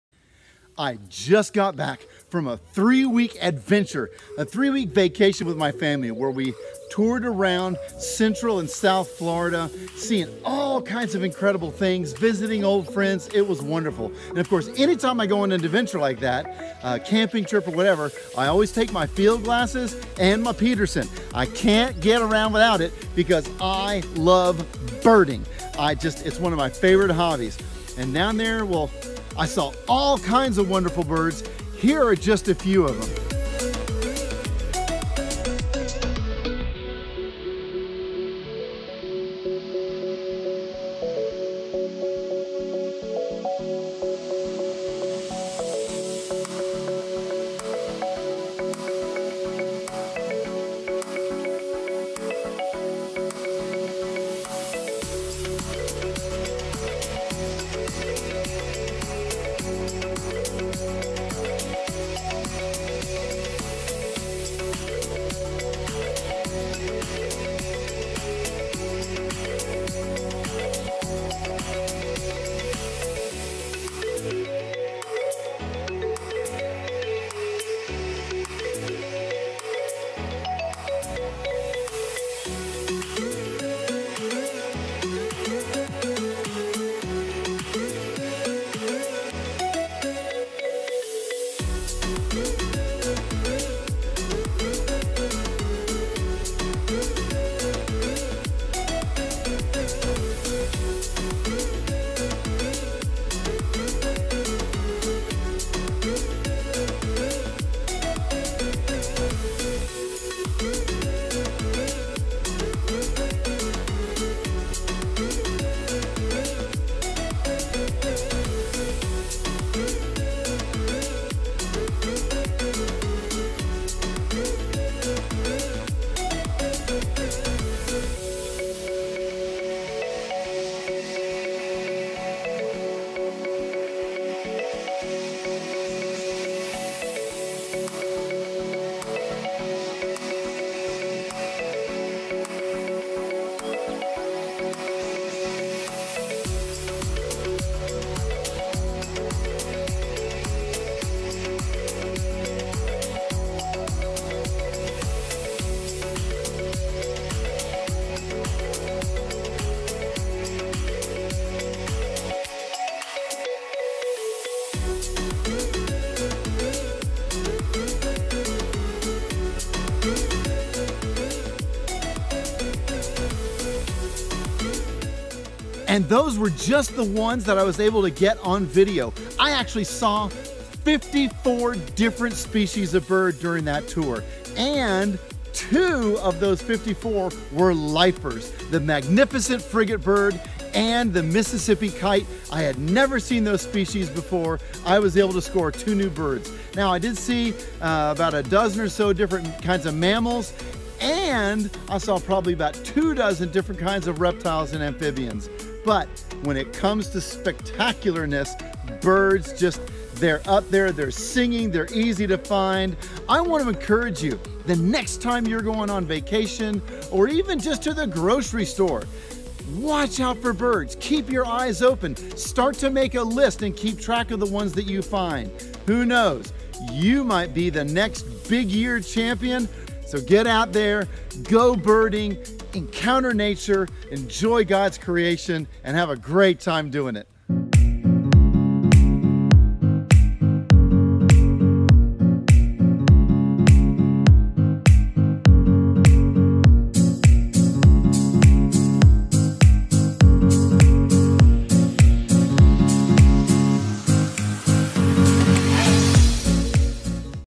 Florida Birding
In this video Gregg Thompson shares his love of birding and some video of just a few of the many species he saw down in south and central Florida.  We hope you enjoy seeing these animals as much as Gregg enjoyed filming them!